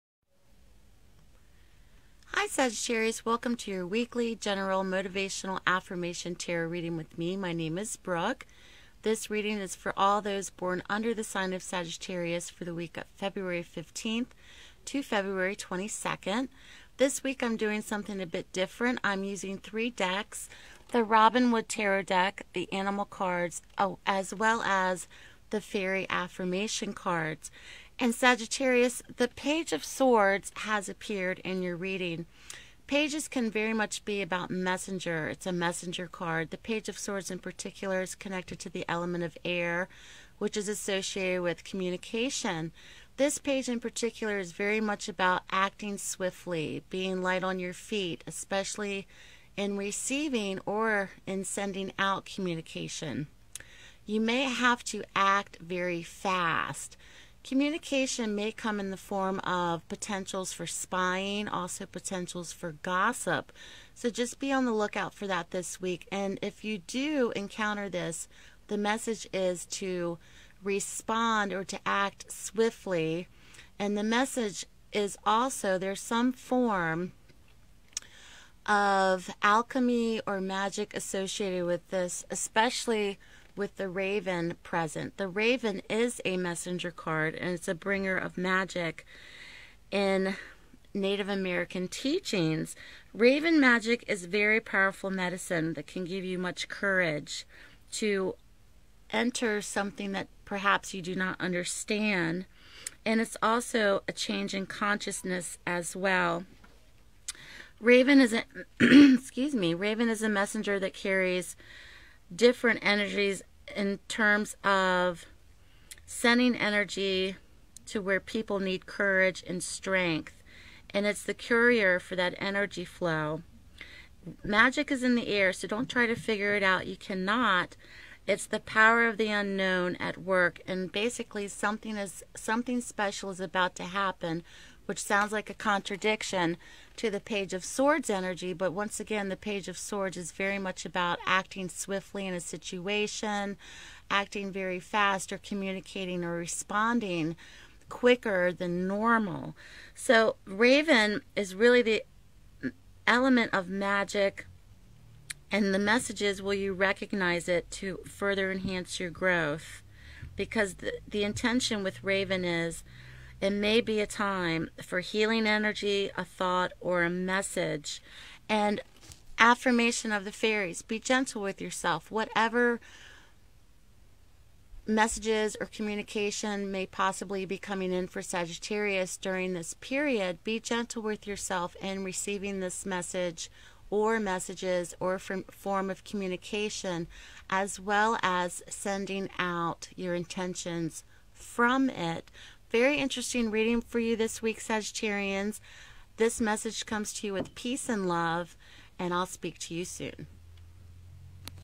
SAGITTARIUS - MOTIVATIONAL/AFFIRMATION GENERAL TAROT READING FOR FEBRUARY 15-22
This is an enlightening reading to send messages of support from my spirit guides.  This is a GENERAL reading for all those born under the sign of SAGITTARIUS.  Please watch for your sun, rising and moon sign.  These messages are for you!